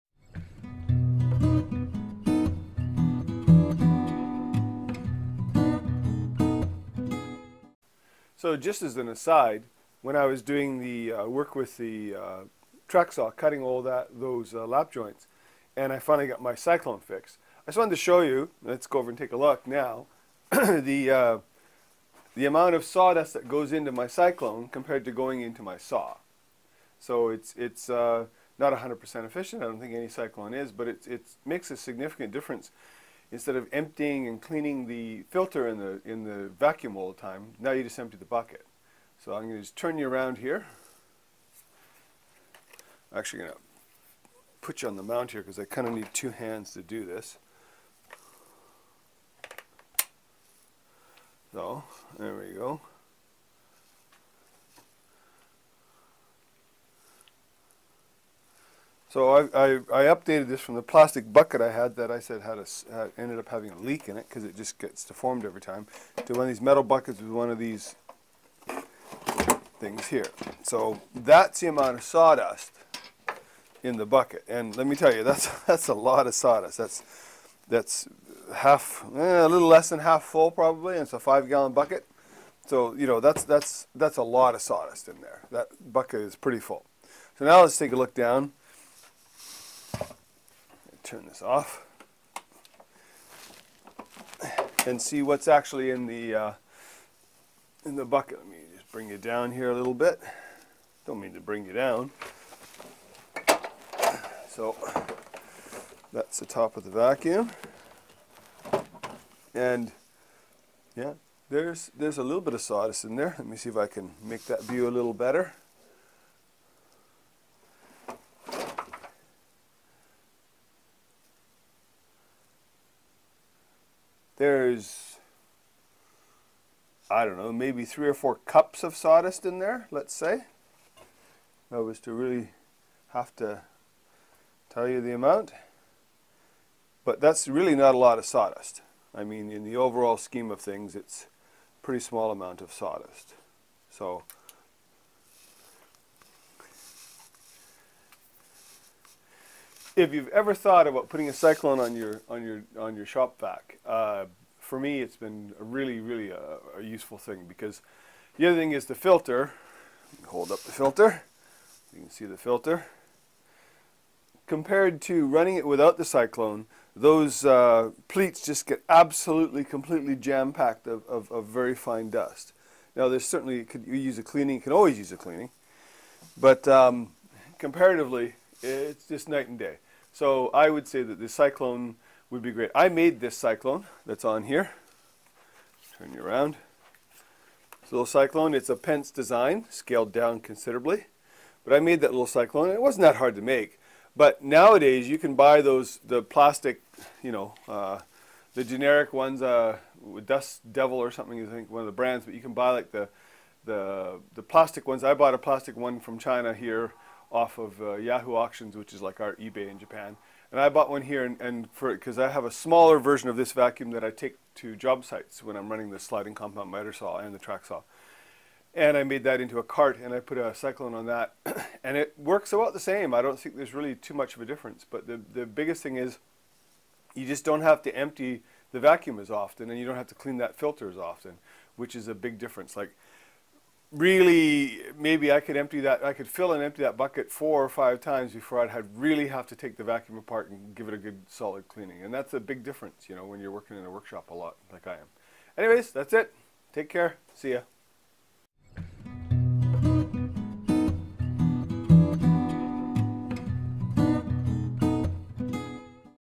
ShopVac Cyclone Upgrade
Just a quick Vlog on upgrading my cyclone on my ShopVac